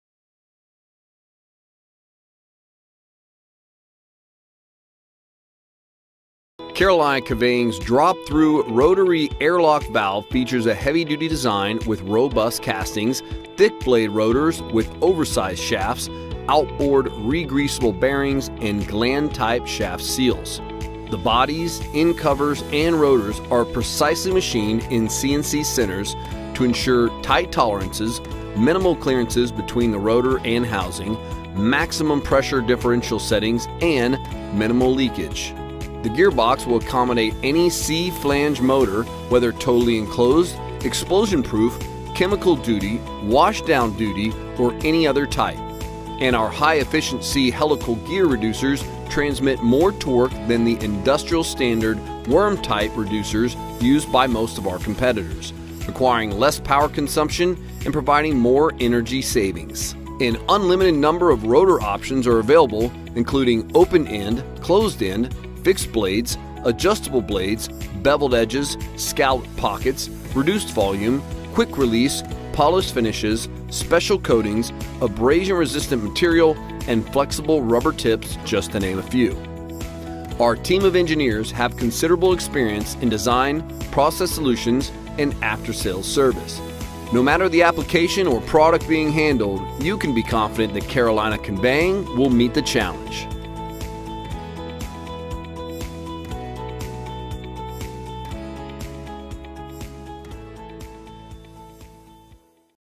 Rotary Airlock Valves for Material Feeding and Pneumatic Conveying
Rotary Valves are used in Feeder applications to feed products from a bin or hopper at a controlled rate. Rotary Valves are also used in Pneumatic conveying applications to feed material into a system against pressure or vacuum.